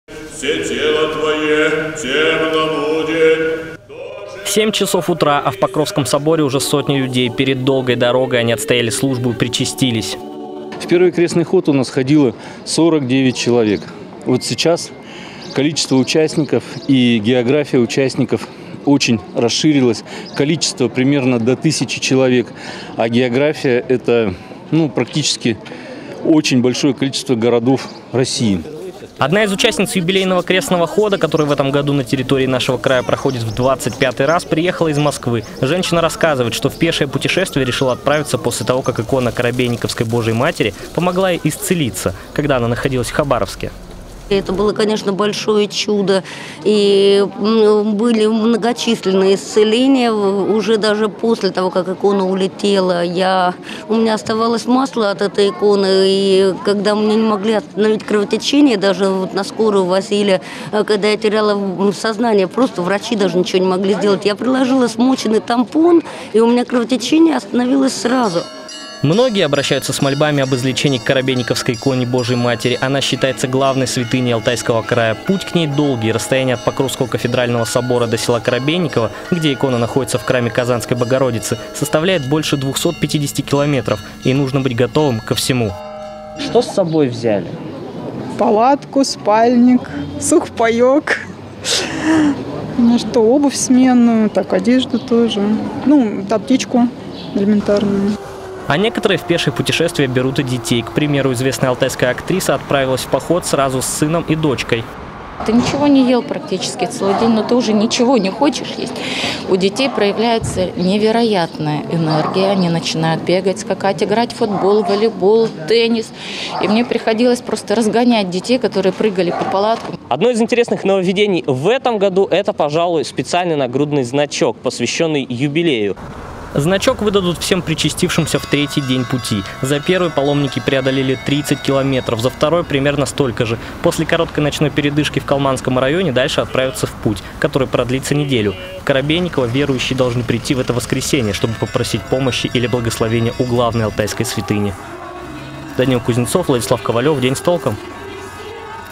Тысячи паломников со всей России продолжают крестный ход в Алтайском крае
Накануне крестный ход, по традиции, начался у Покровского Собора в Барнауле и завершится в Усть-Пристанском районе, где находится коробейниковская икона Божией матери.